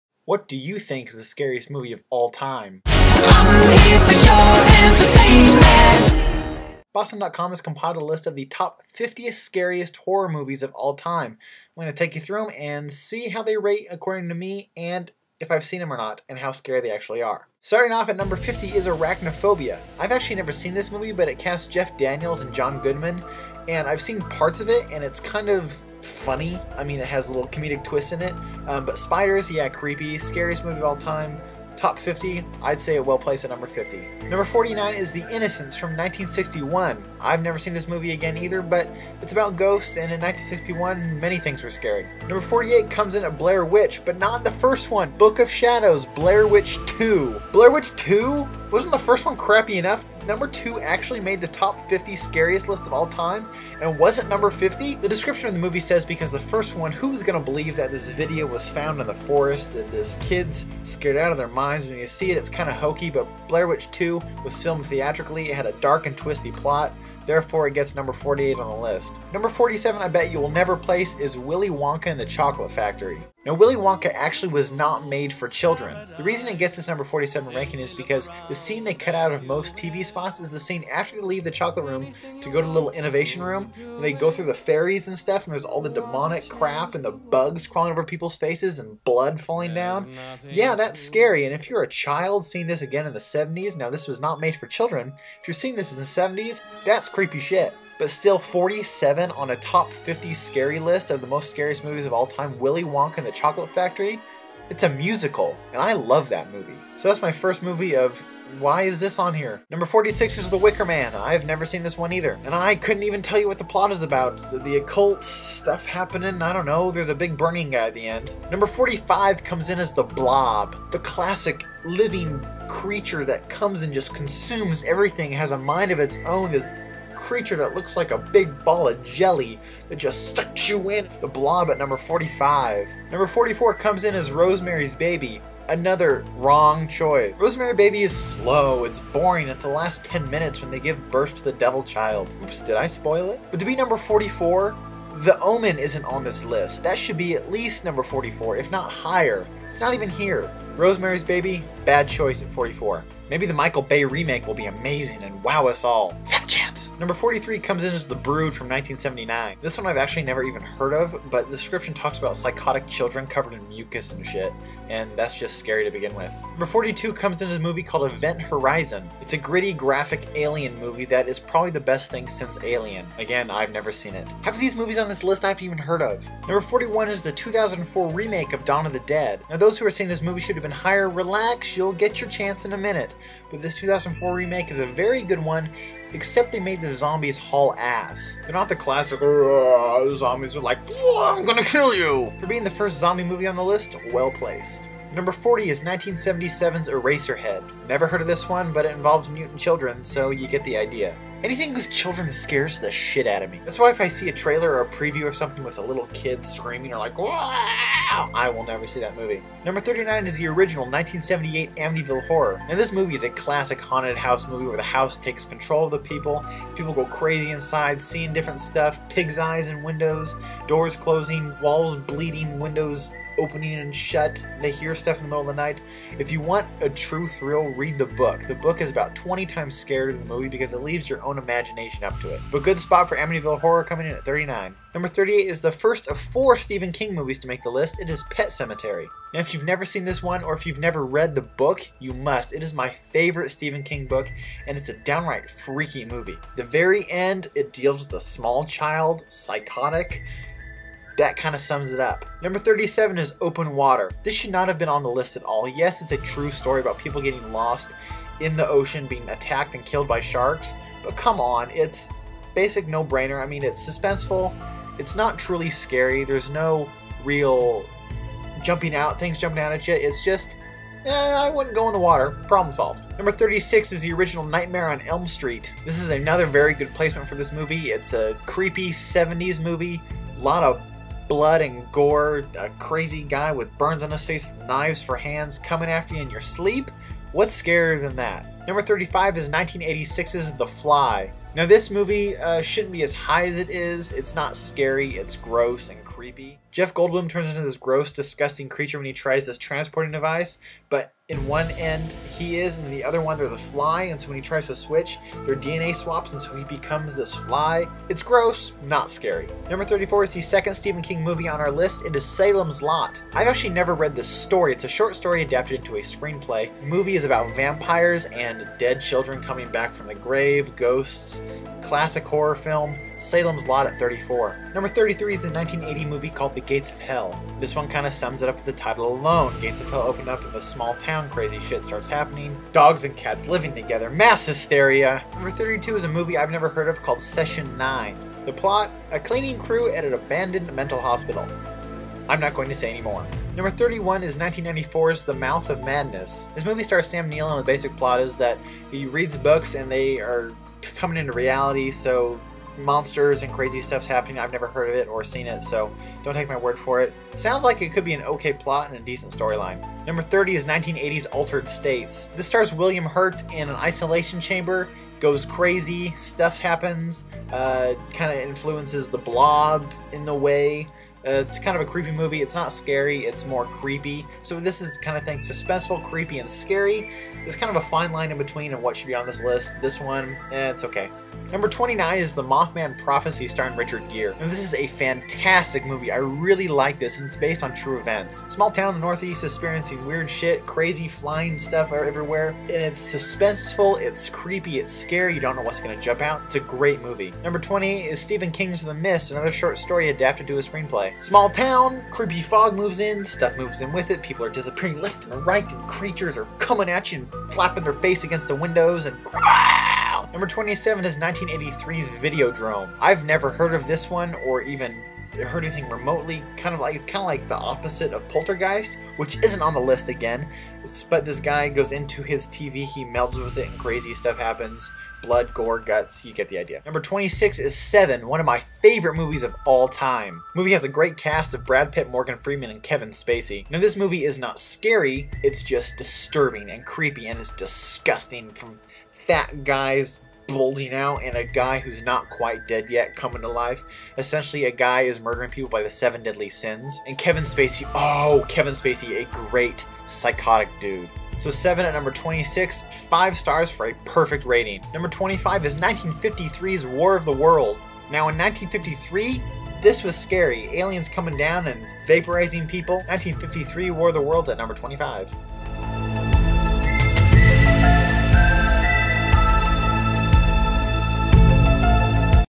50 Scariest Movies of All Time (Part 1)
Counting down numbers 50-25 of the scariest movies of all time